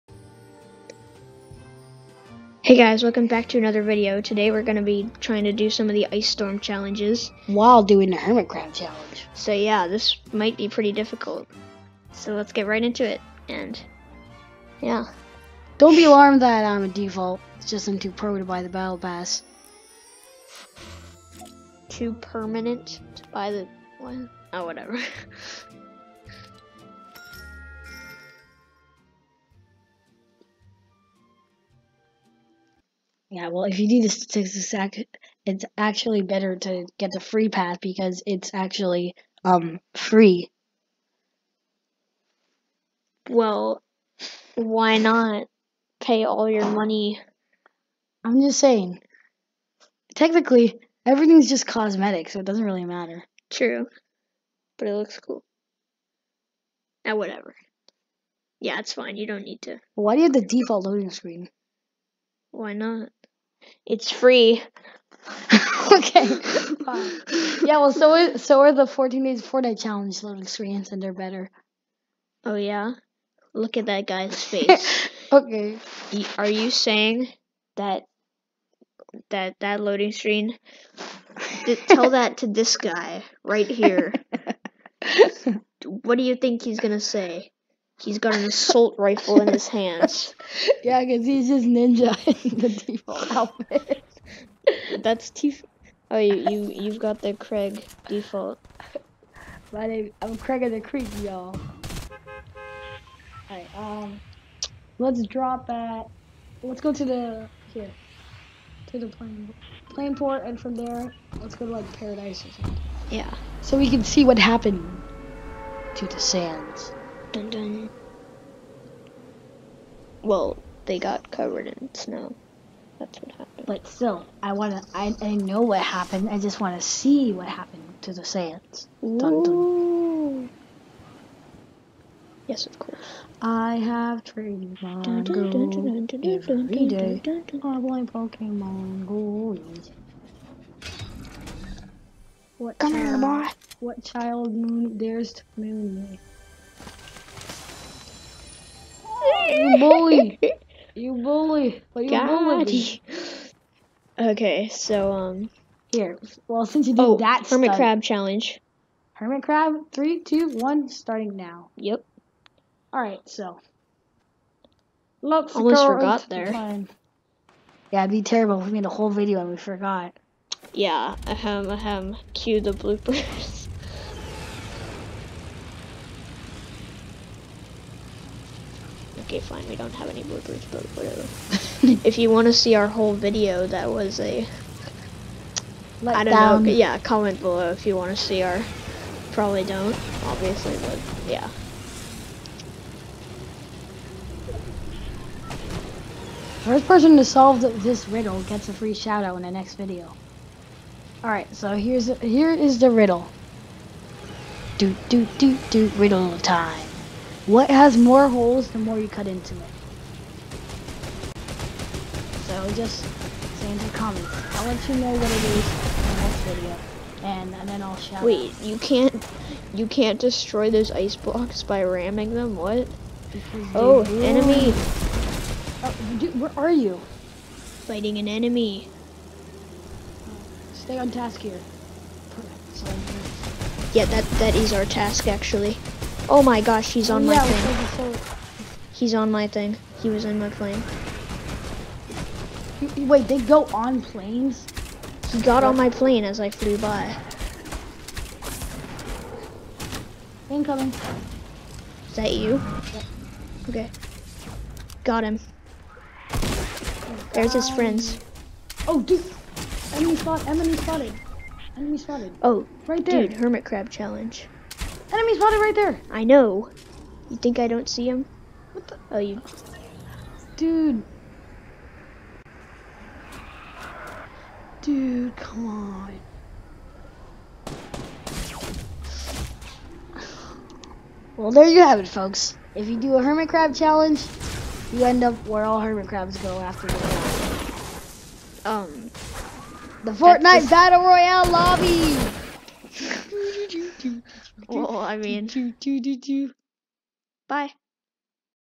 Planes in a nutshell
we try to do the hermit crab challenge but end up just using planes